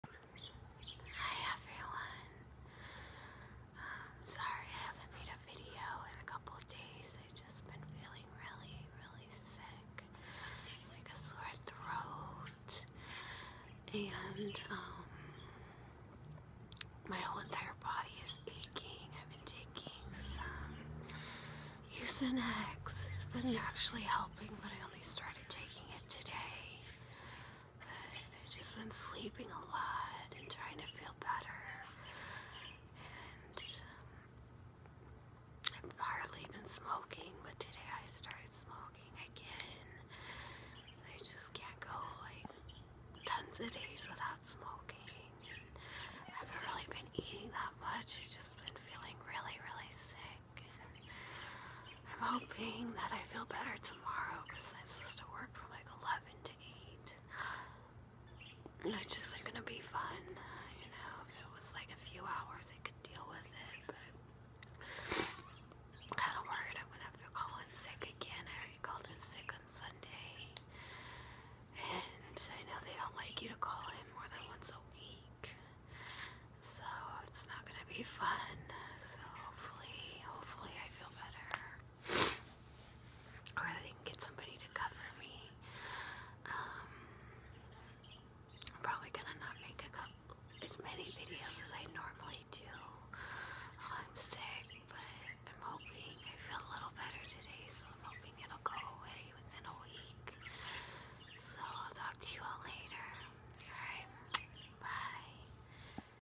ASMR Rambling Update Video Vlog
In this ASMR video I will be rambling on my updates in a soft spoken voice. This video was meant to relax you and bring you tingles. Let me know what you think of it!!